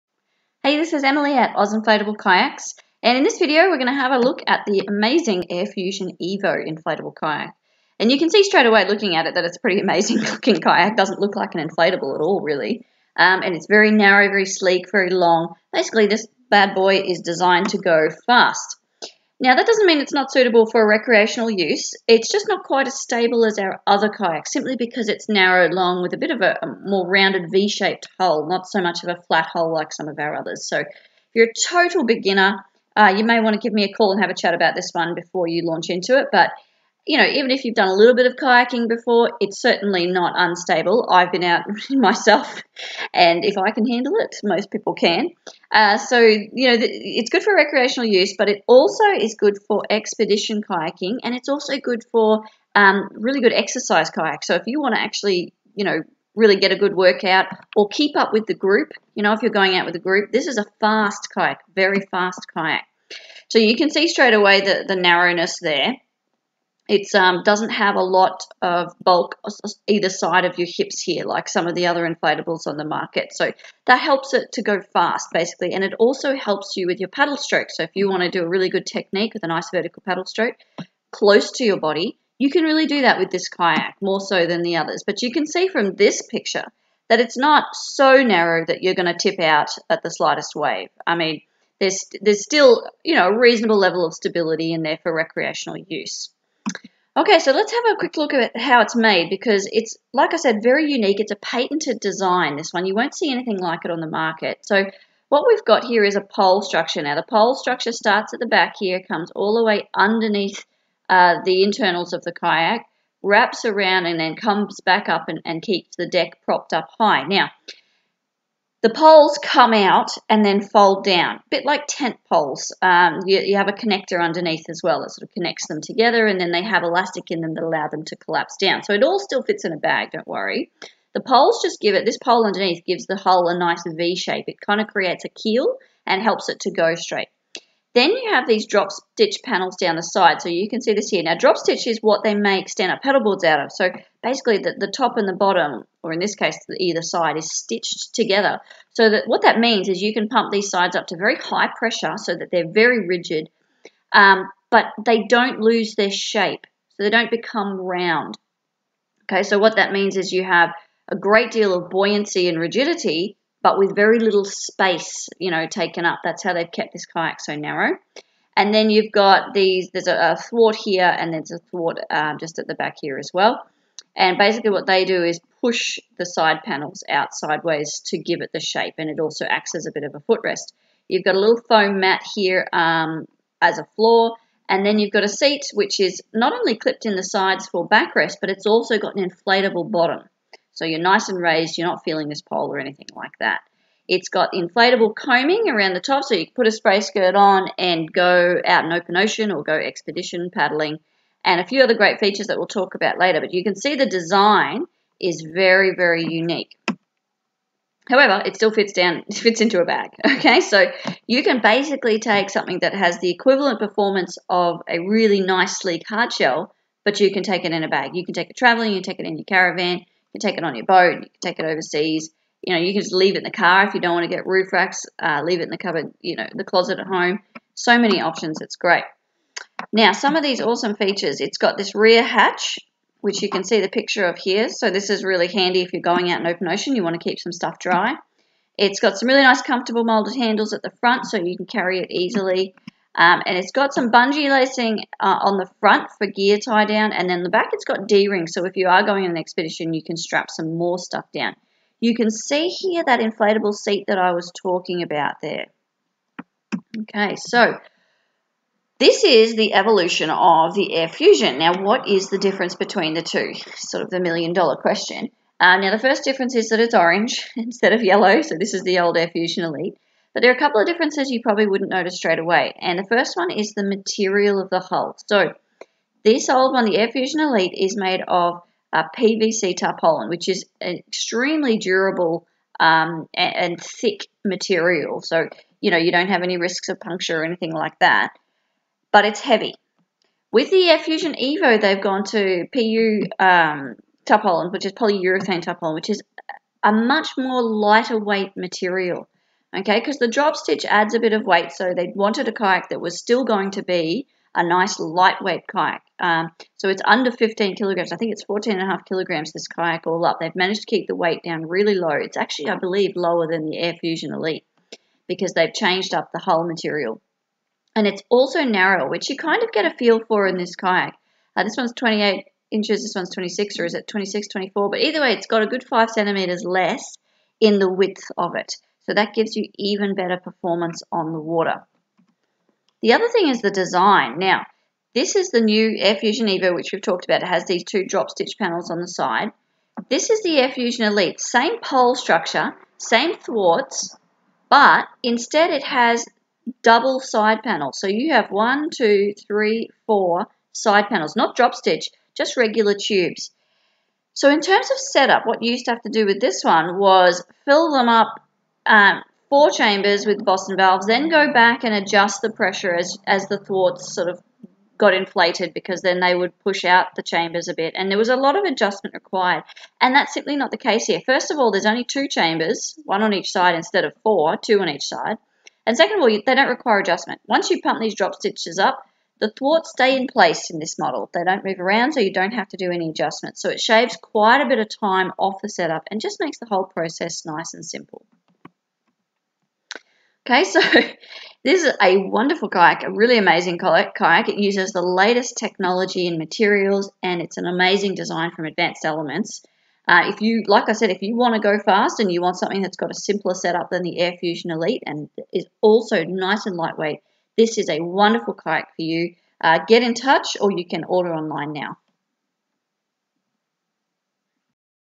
AirFusion Evo Inflatable Kayak from Advanced Elements AE1042-O
Here it is! The latest and greatest inflatable kayak from Advanced Elements - the amazing AirFusion EVO.

This video will help you to decide if the AIrFusion EVO inflatable kayak is right for you.

There is also a section that explains the difference between the original AirFusion Elite and the new AirFusion EVO inflatable kayaks.

Basically, Advanced Elements have taken the latest drop-stitch technology to reduce the set up time and give this speedy kayak an even more streamlined hull. They have also used polyurethane tarpaulin instead of the traditional PVC tarpaulin to keep the kayak at a super lightweight 14.5kg.

Great job Advanced Elements, another awesome inflatable kayak!